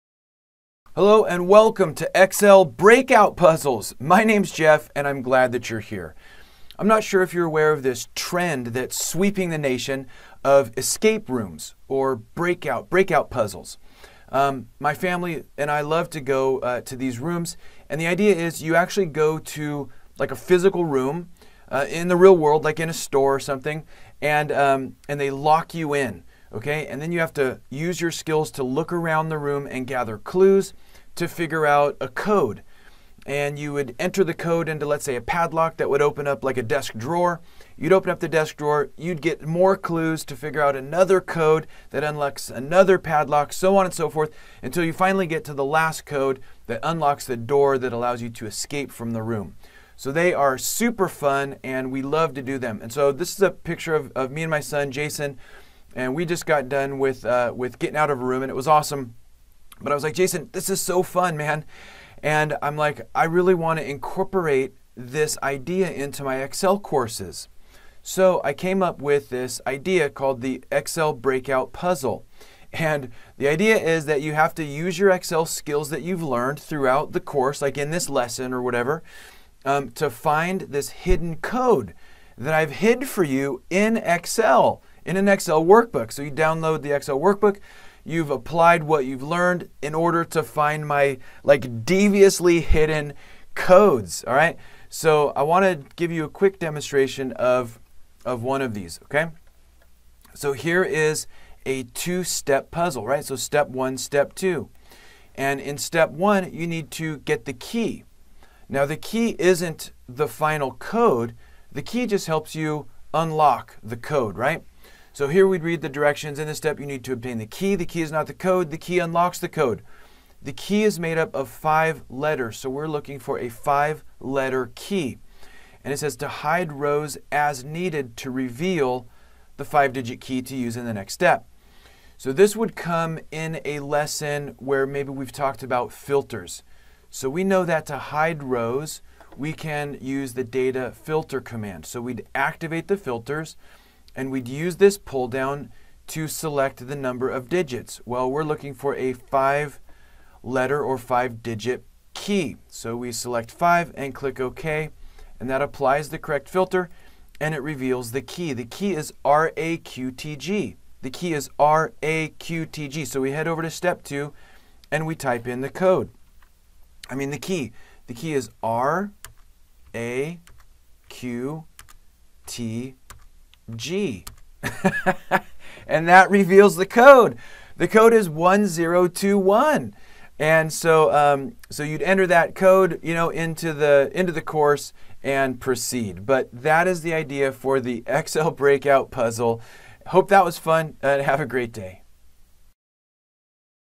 Excel Breakout Puzzles!
Are you aware of the Escape Room trend, or the similar breakout logic puzzles that teachers use in classrooms? Here is the basic idea of the escape room.

You and some friends go to a company that has created an escape room. You get locked into the room, and the timer starts. Before the timer goes off, you must gather clues and solve puzzles to obtain the code to the lock in order to leave the room, or escape. There are typically several codes and padlocks. So, you look around the room and solve a puzzle to get a code. You enter the code into a padlock on a drawer, small safe, or lockbox, to open it. Inside, you’ll find clues that help you get the next code, which opens another lockbox, and so on, until you ultimately figure out the code that allows you to escape the room. Our family loves them.

A similar version is played in schools as well, typically in math or STEM classes. The teacher will create breakout puzzles around the classroom. Students must find clues to figure out the codes. Entering the correct code unlocks a box. The puzzles require a bit of math and logic to get the code. It is a great learning activity, and helps the students stay engaged and have a bit of fun while applying what they learned.

I was telling my son, Jason, that I wanted to create something similar for my Excel courses. So, I’ve created a bunch of Excel Breakout Puzzles…how fun is that! You must use the Excel skills presented in the course to solve the puzzle and enter the code. Basically, I hide the code somewhere in the Excel workbook, and you have to use what you learned to reveal the code.

These Excel breakout puzzles are incorporated as a learning activity in my Excel University Graduate Certification program.

If you'd like a copy of the Excel Breakout Puzzle demonstrated in this video, just head to the Excel University blog.